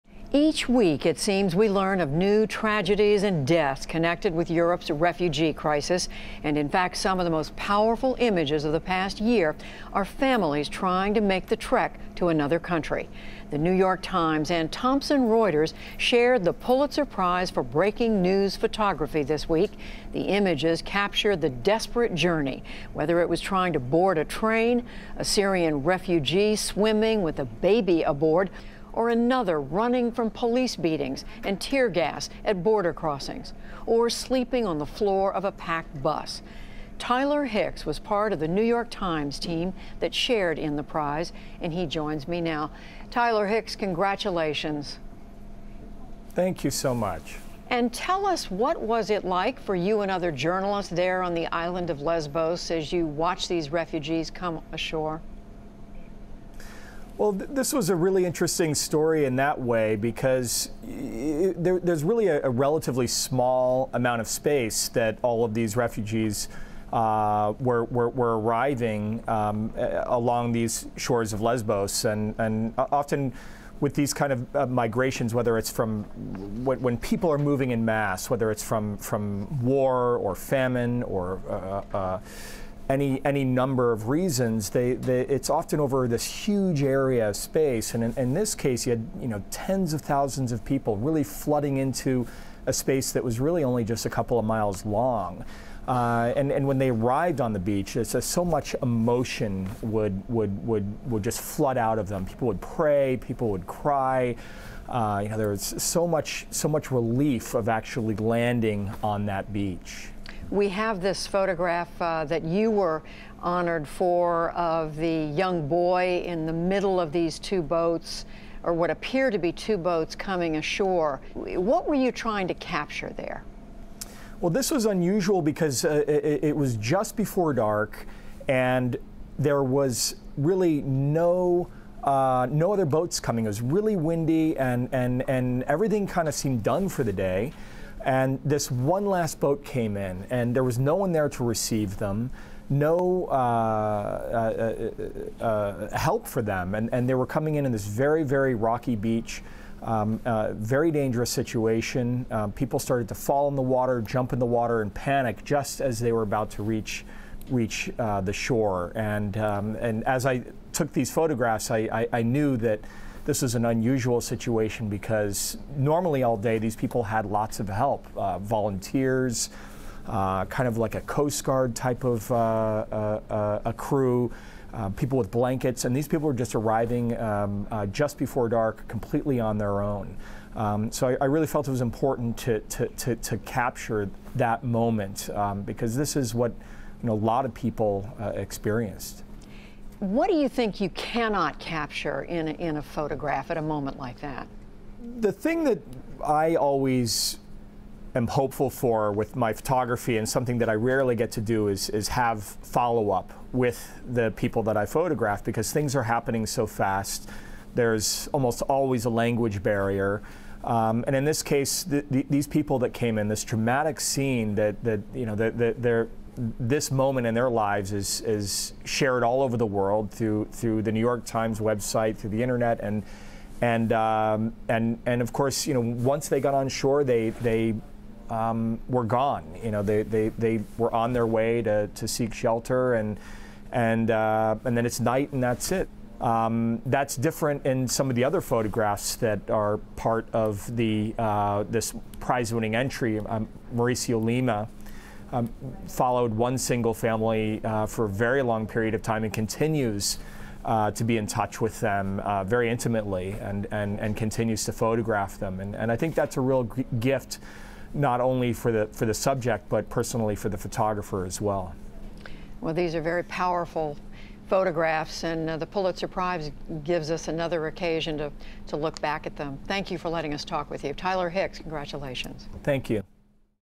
Pulitzer-winner Tyler Hicks on capturing the migrant crisis
New York Times photographer Tyler Hicks was part of a group of photojournalists that was awarded the 2016 Pulitzer Prize for Breaking News Photography for their coverage of the European migrant crisis. Hicks previously won the same prize in 2014 for his coverage of the terror attack on Kenya's Westgate shopping complex. In a conversation with the NewsHour, Hicks describes his time documenting the experiences of refugees landing on the Greek island of Lesbos.